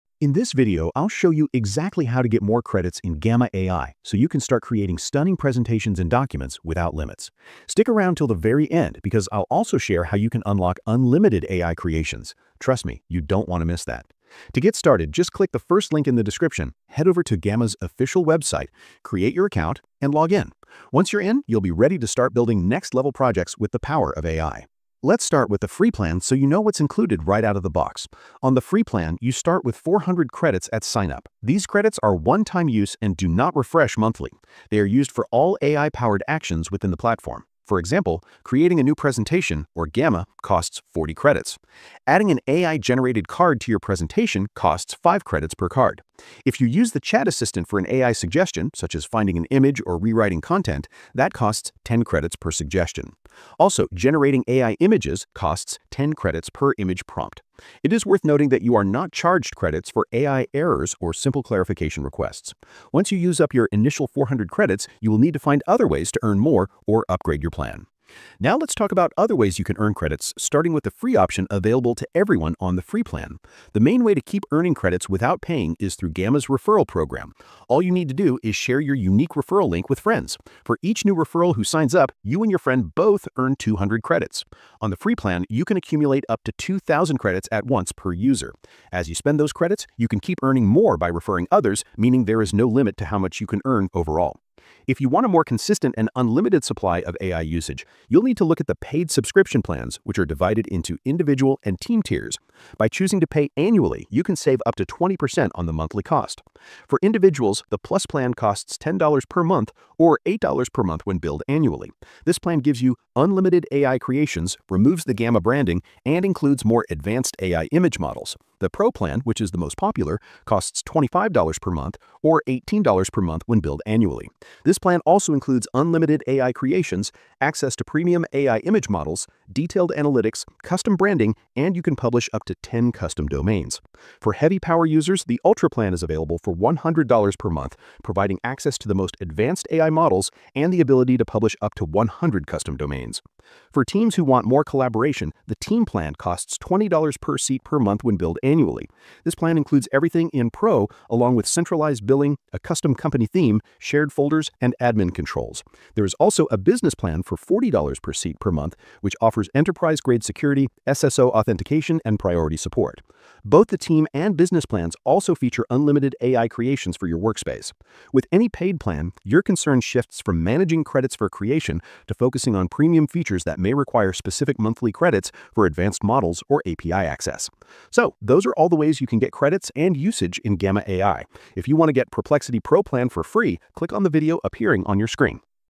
How to Get more Credits in Gamma AI | Gamma AI Pricing Plans Explained
Wispr Flow is my tool of choice for fast, polished voice-to-text in any app

In this video, I’ll guide you through the process of earning more credits in Gamma AI, enabling you to create stunning presentations and documents without limits. I’ll cover everything from the free plan to referral programs and paid subscriptions, ensuring you maximize your AI usage. Stick around till the end for tips on unlocking unlimited AI creations!

Timestamps
00:00 Intro to Gamma AI Credits
00:02 How to Start Earning Credits
00:26 Overview of Free Plan Benefits
01:08 Earning Credits Through Referrals
01:35 Paid Subscription Plans Explained
02:43 Team and Business Plan Features
03:01 Summary of Earning Credits